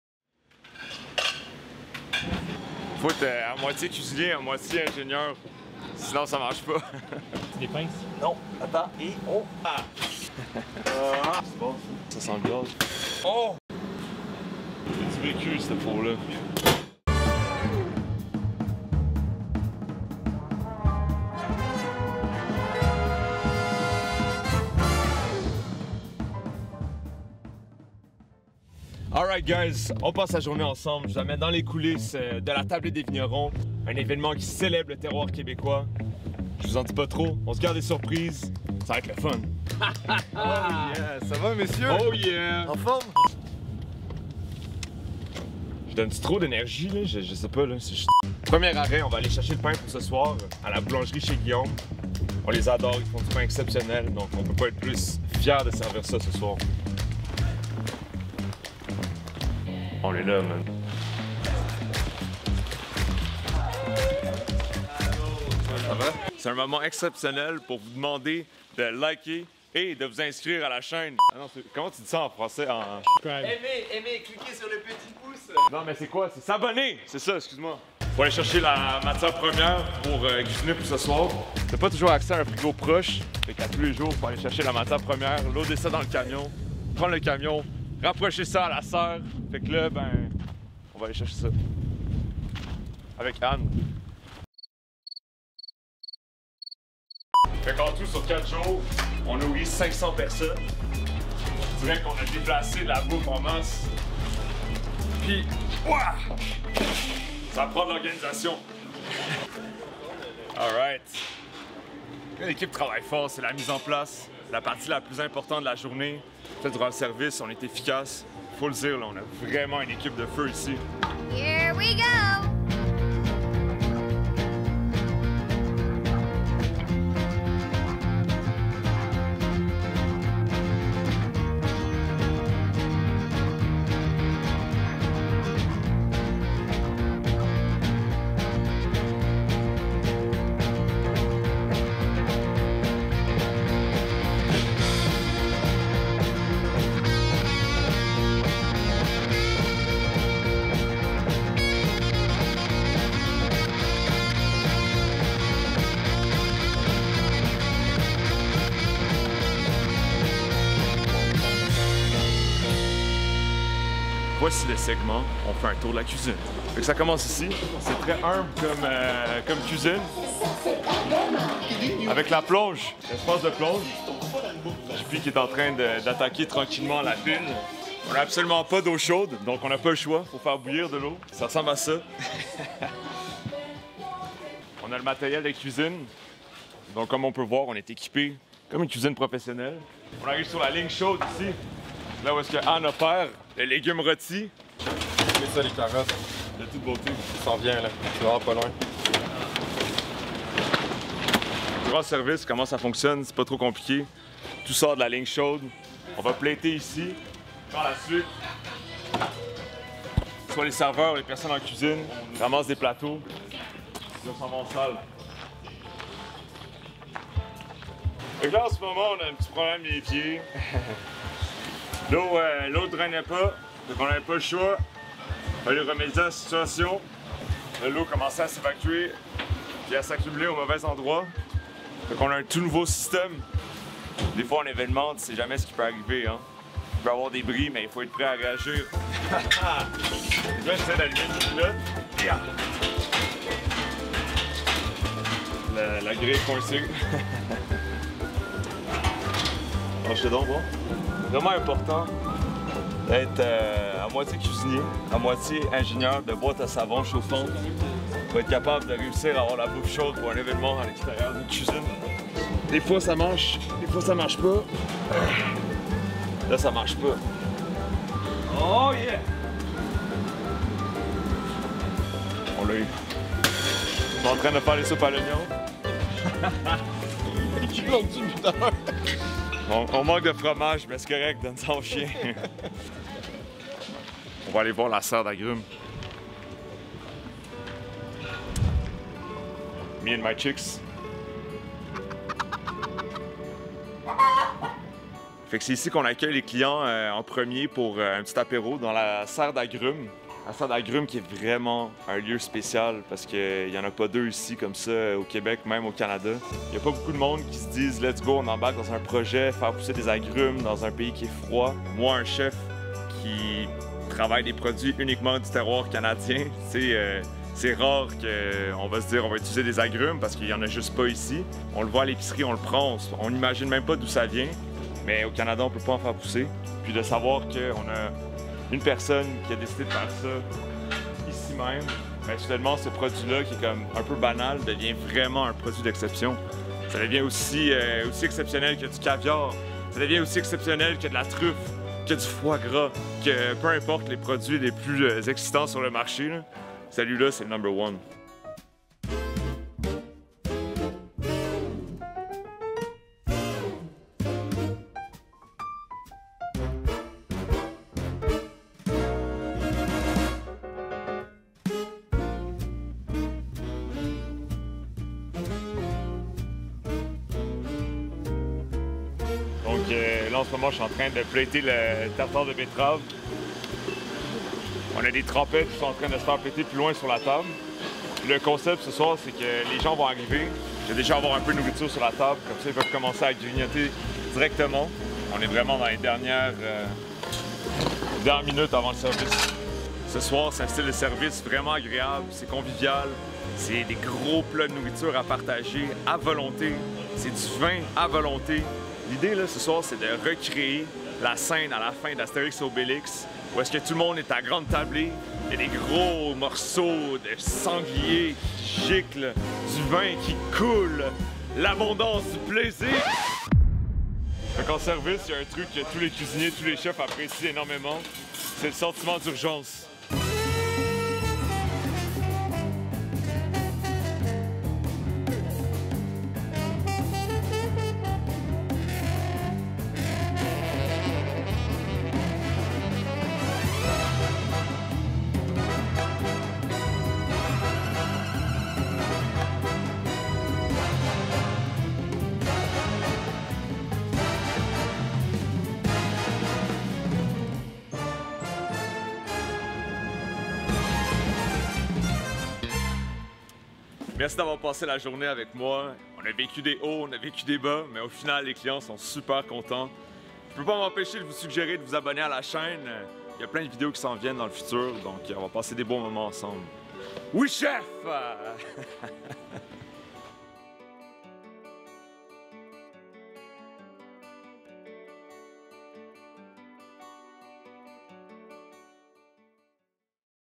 Un Événement Gastronomique à la Ferme.
Suivez-moi dans les coulisses de La tablée des vignerons ; un événement gastronomique à la ferme Vaillancourt.

Videographe  ►  Justinbissonbeck.com

Mots clef:
Cuisine, recette, Camilo Lapointe-Nascimento, chef, gastronomie, Quebec, table champêtre, pop up,